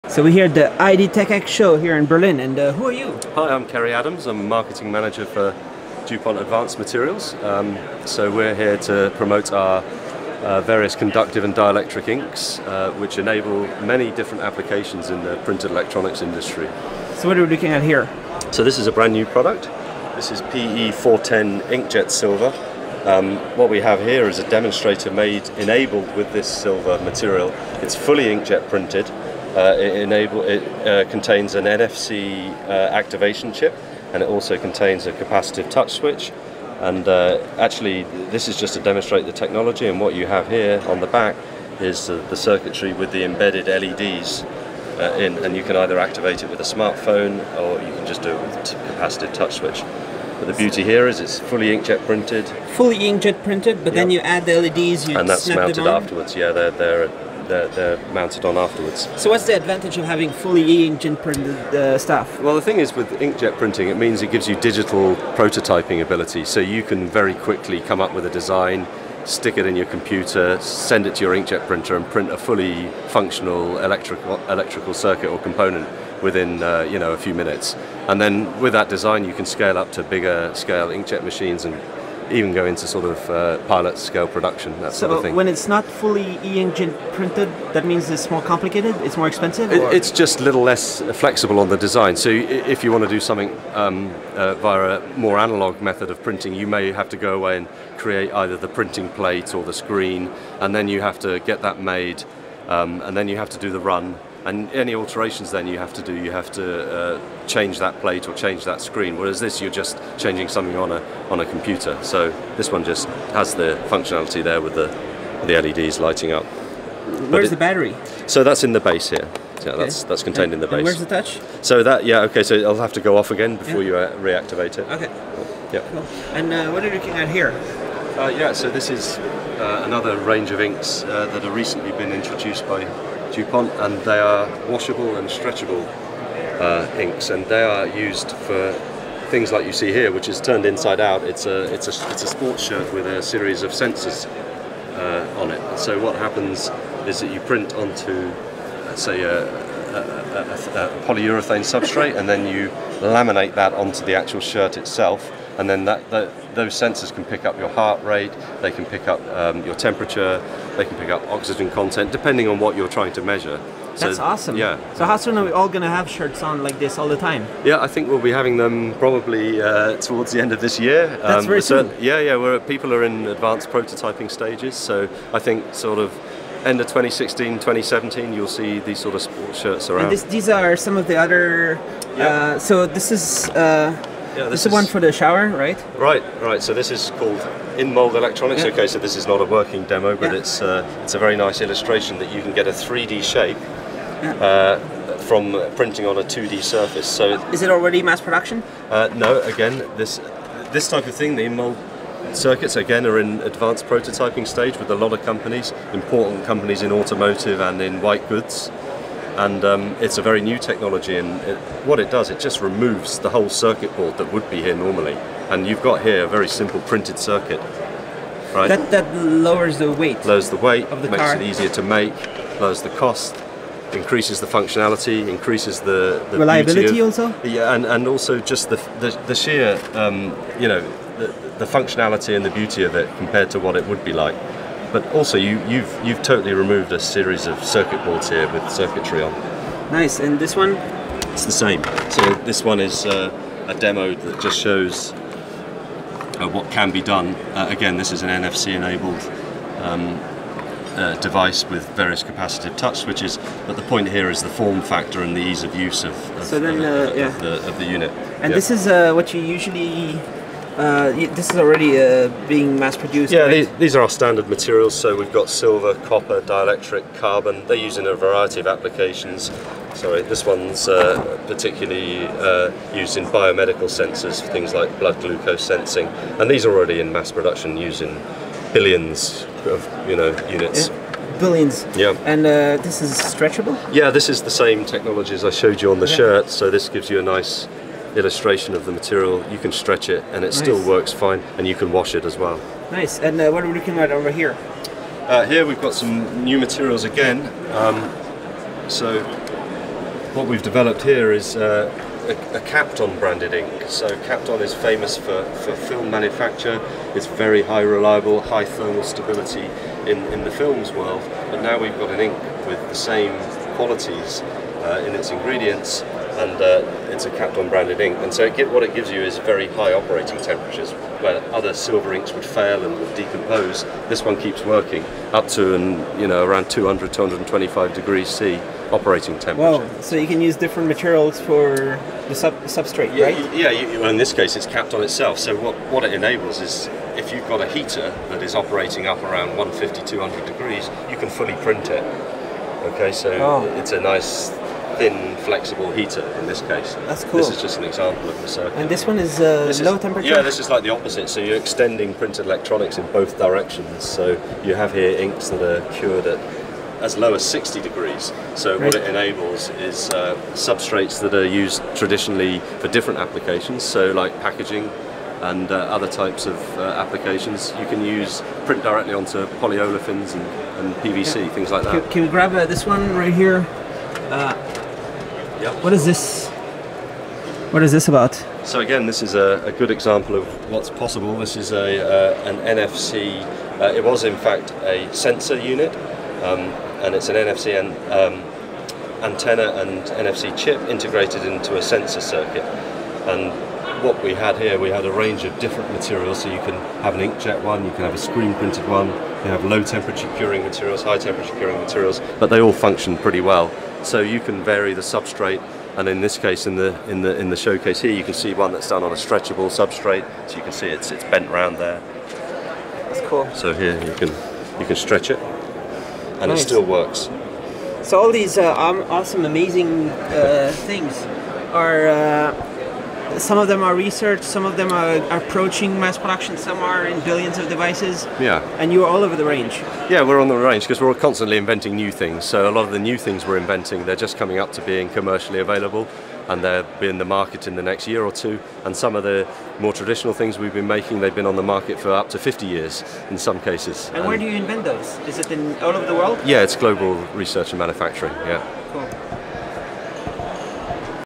DuPont PE410 ink for rapid prototyping of flexible OLED, solar, antennae and touch panels
DuPont Advanced Materials (DuPont) launches a new electronic ink for inkjet printing that offers the high conductivity and strong adhesion required for rapid digital design, prototyping and full-scale manufacturing.  The technology will enable digital printing for electronic components and circuits in applications where extremely fine lines are required, such as OLED panels, solar cells, printed antennae and touch panels.

DuPont’s newest conductive ink, PE410, enables rapid prototyping and a smooth transition from “lab to fab” with the versatility to scale up to industrial high-volume inkjet print heads and machines. This allows circuit designers to immediately test a new design, quickly make necessary edits, and, due to reduced silver laydown, save on material costs.  The technology also can be adapted to non-planar printing, enabling a series of new and emerging applications.

Stretchable inks for wearable electronics that provide a manufacturing-ready alternative to traditional methods of embedding electronics in clothing and are used to create thin, form-fitting circuits that can be seamlessly bonded with many standard fabrics.

A suite of in-mold electronic inks which help create lighter, less expensive and more beautiful electronic devices by reducing the need for rigid circuit boards.  These inks enable circuits to be printed directly onto plastic substrates and allow electronic features such as electronic controls, capacitive switches and LED lighting, to be readily integrated in applications such as home appliances and automobiles.

New low-temperature inks that cure quickly at temperatures as low as 60C, opening up the possibility for printed electronics designers to use a wide range of functional and low-cost plastic films.